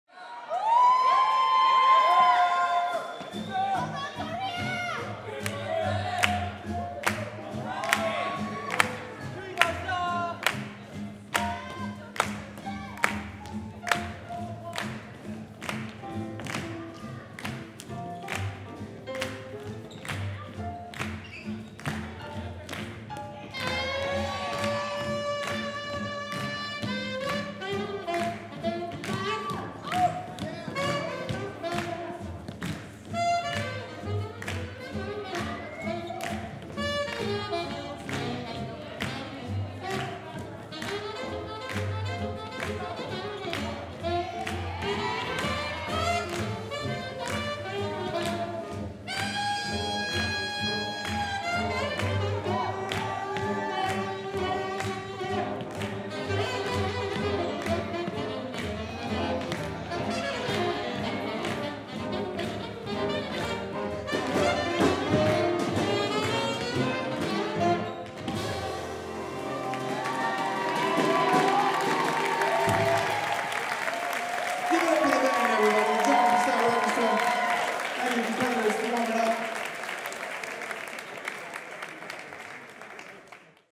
ILHC 2018 - Open Strictly Lindy Hop Finals - All Skate
Taken at The International Lindy Hop Championships in Washington, DC.

Editing by Patrick Szmidt